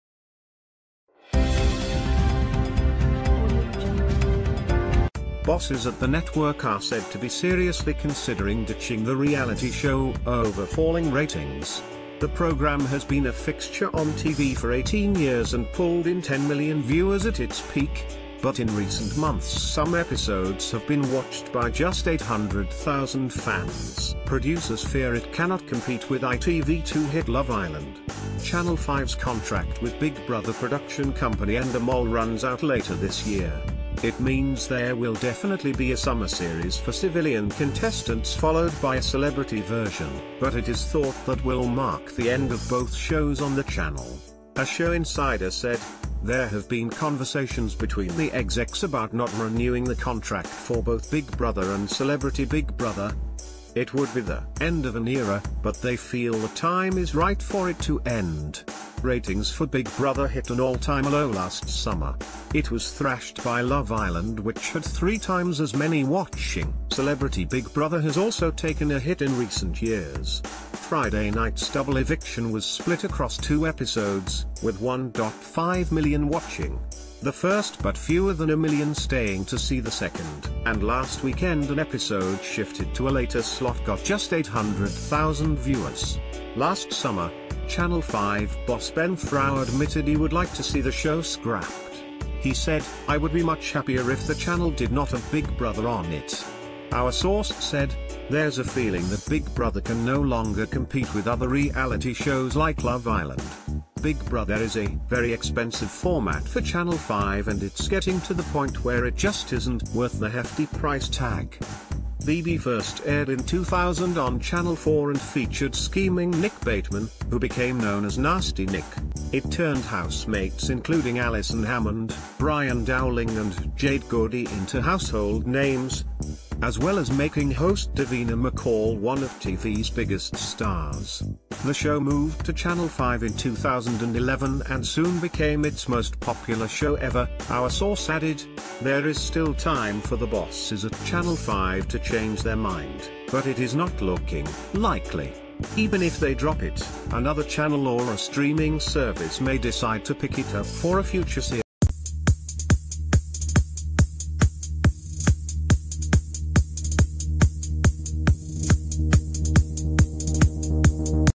Big Brother AXED: End of era as show ed off after 18 years
Bosses at the network are said to be "seriously considering" ditching the reality show over falling ratings. The programme has been a fixture on TV for 18 years and pulled in 10million viewers at its ...